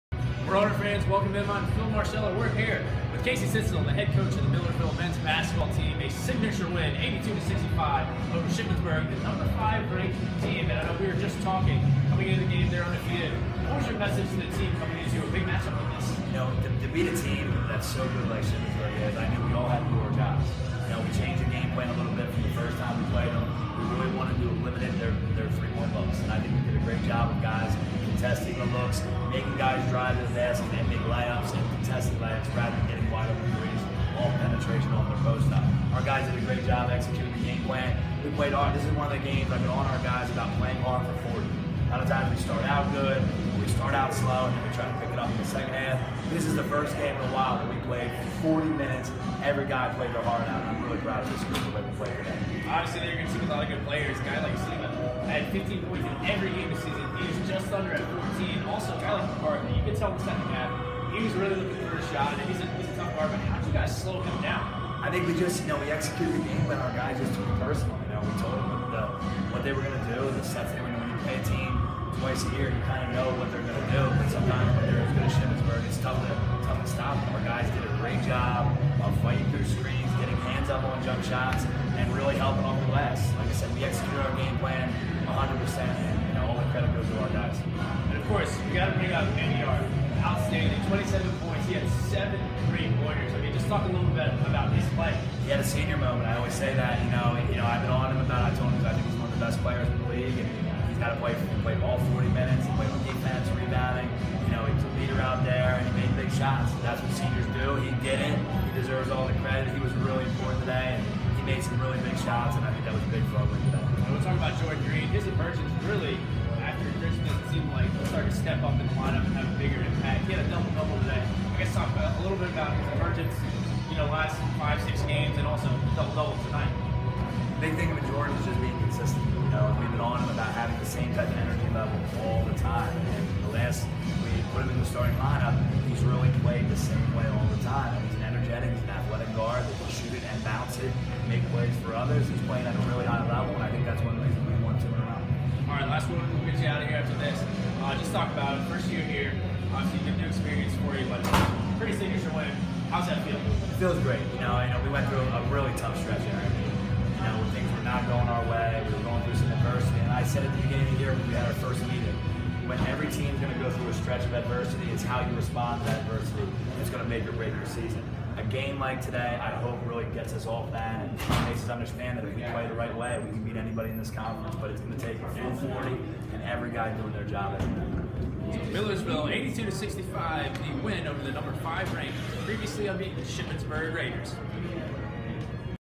Coach Stitzel post-game interview Jan. 28, 2017
Coach Stitzel reflects on Millersville's 82-65 upset over #5 Shippensburg